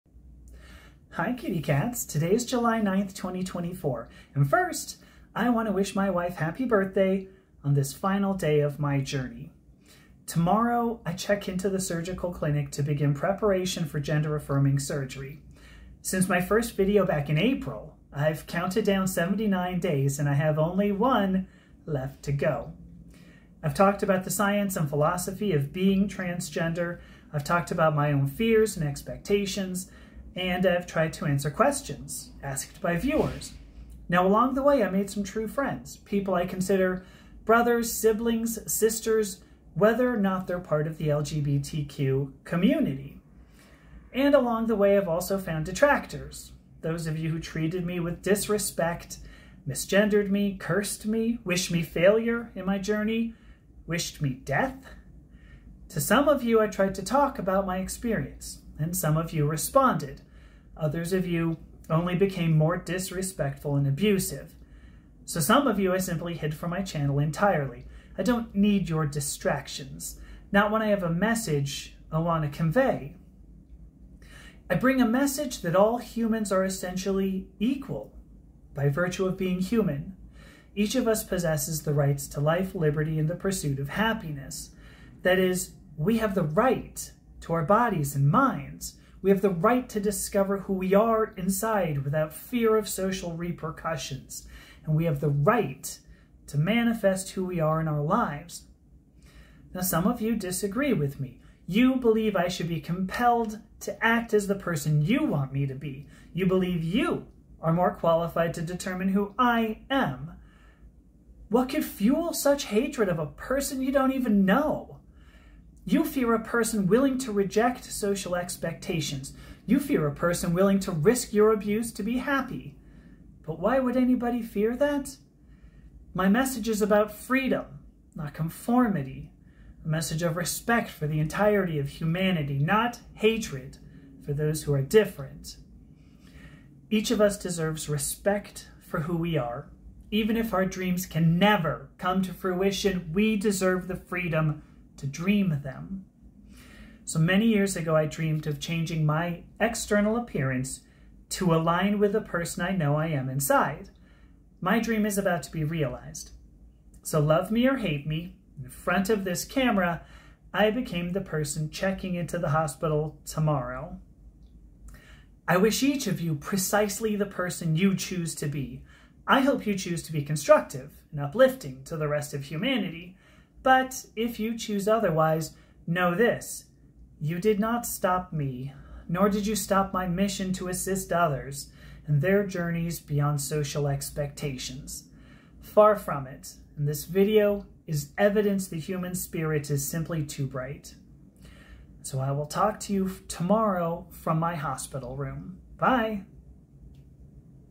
Her Last Message of Equality before She Goes to the Hospital
Today's video is the last before I enter the hospital for gender-affirming surgery, and I want to express that we all deserve the freedom to dream, even if our dreams can never come true.  Humans are all essentially equal.